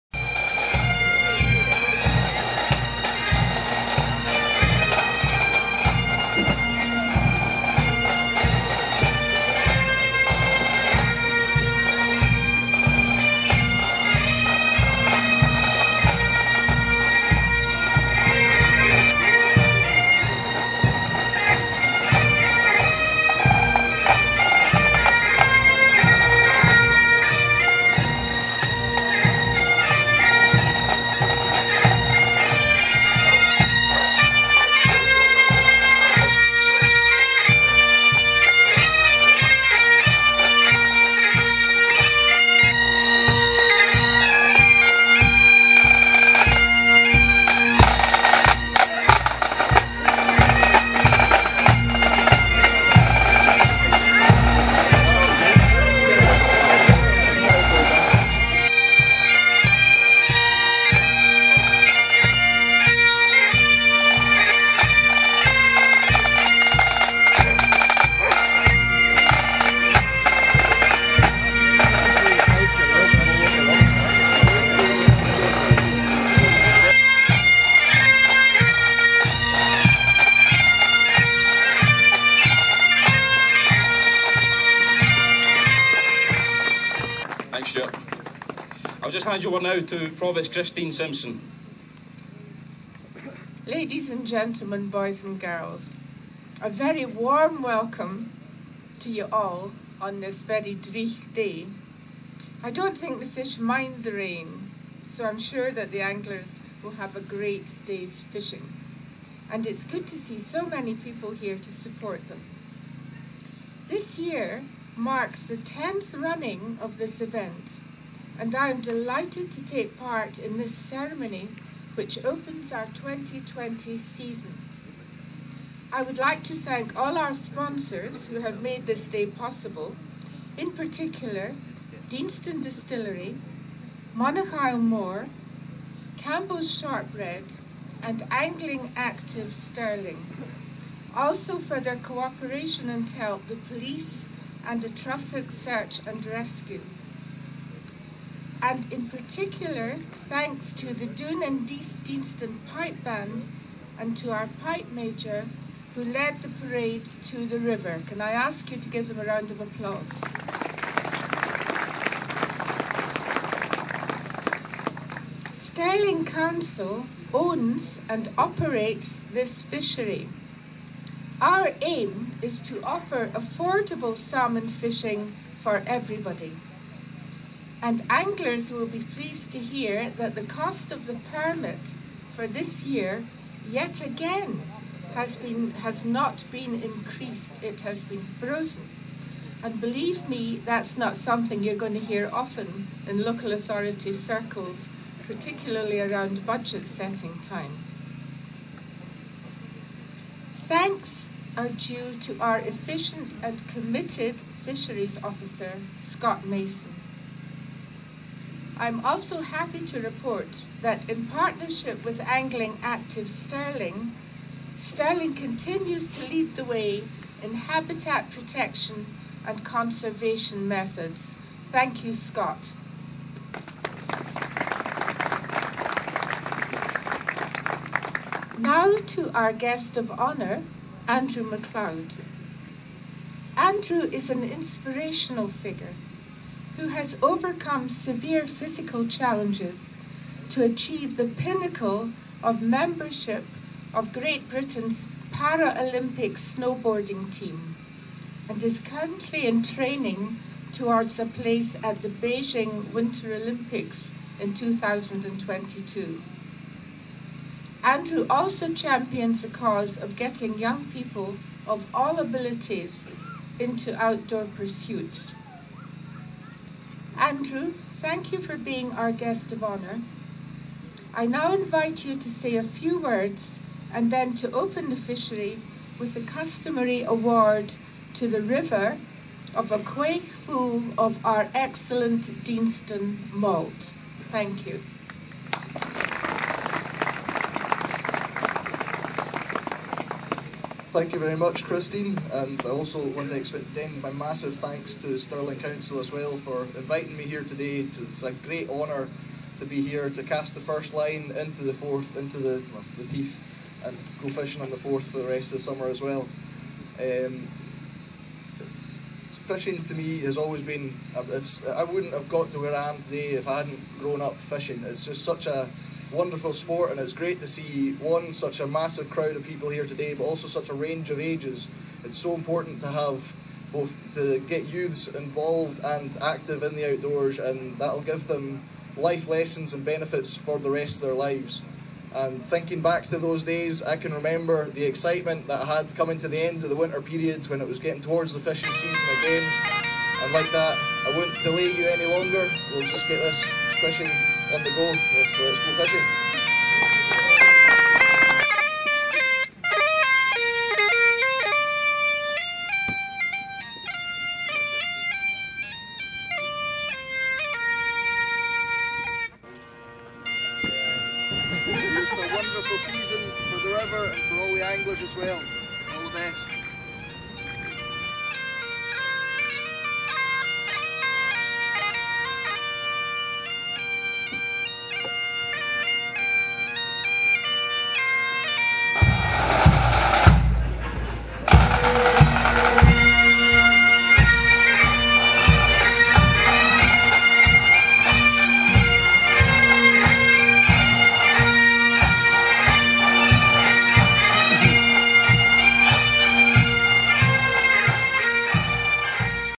Callander Fishing 2020
The Opening of the River Teith Fishing Season Callander 2020